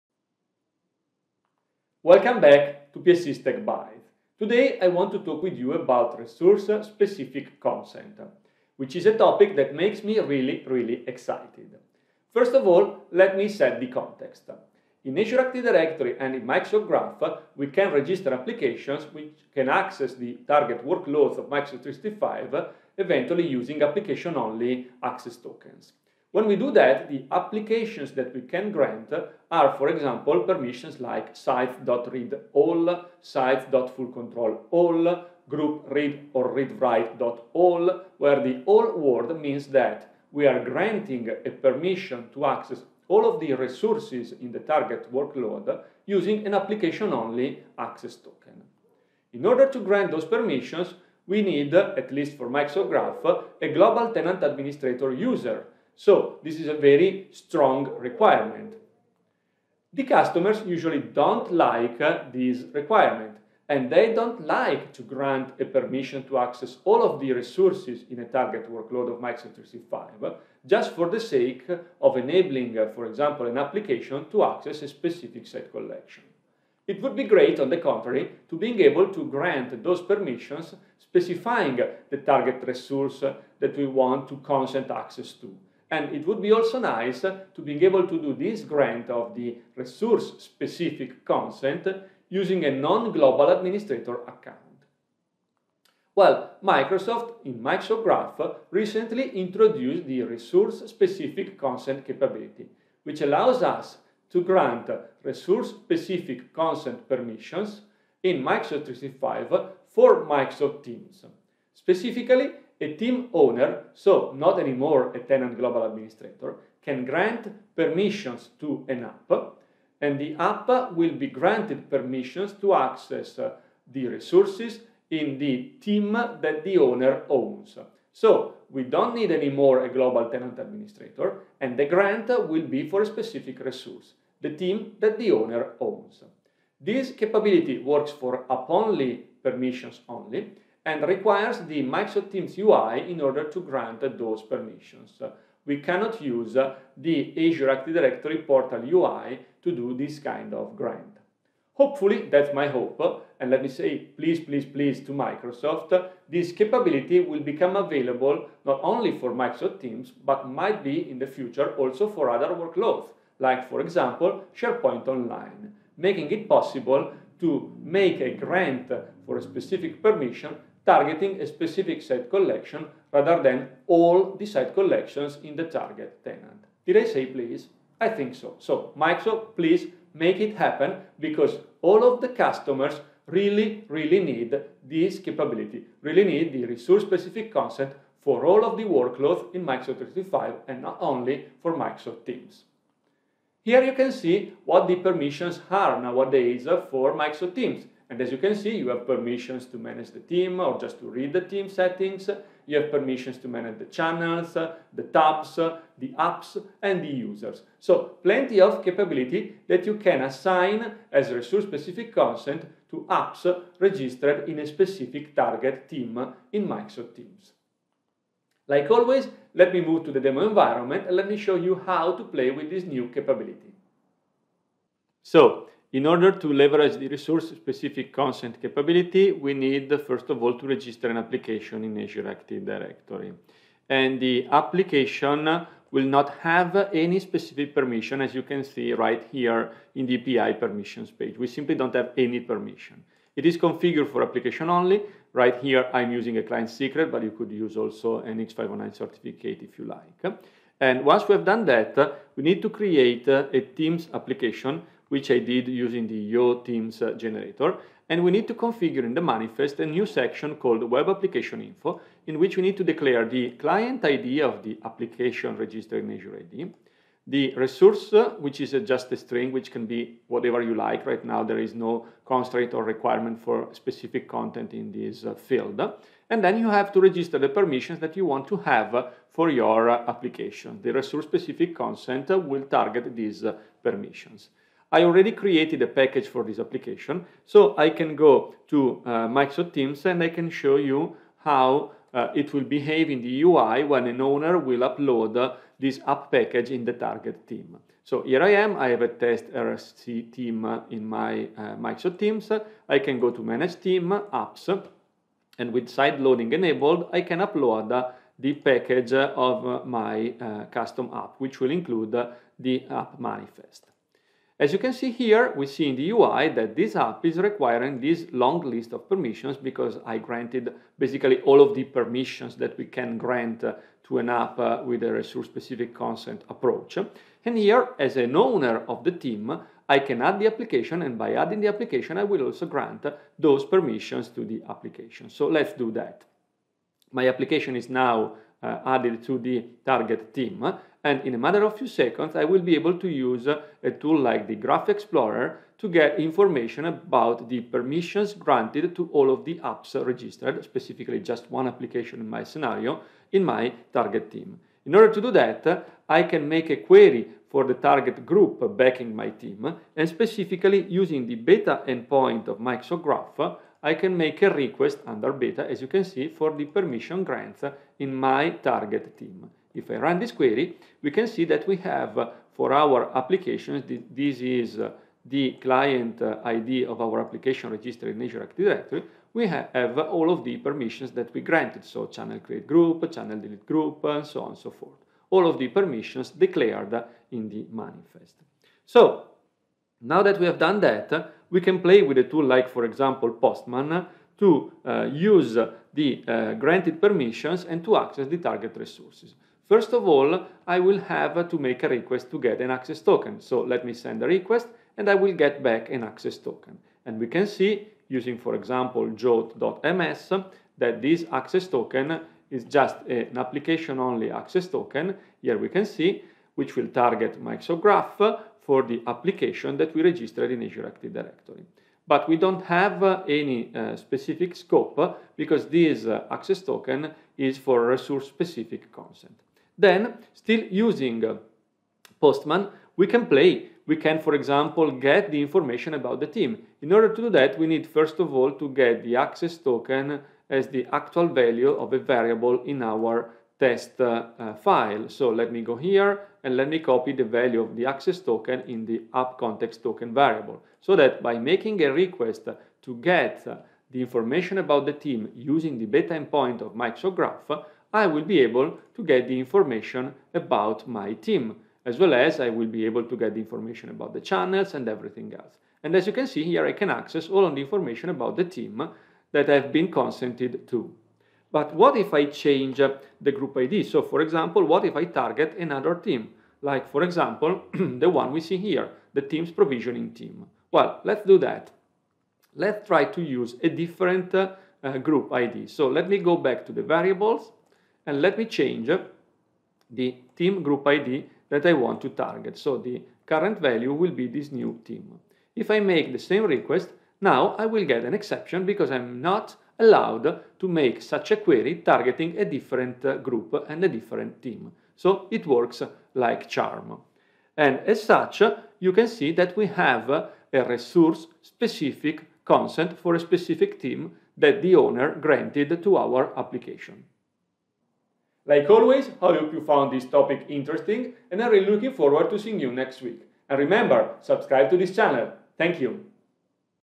Episode #128 - Resource Specific Consent (RSC) with Microsoft Graph and Microsoft Teams
Learn how the Resource Specific Consent (RSC) capability of Microsoft Graph and Microsoft Teams works.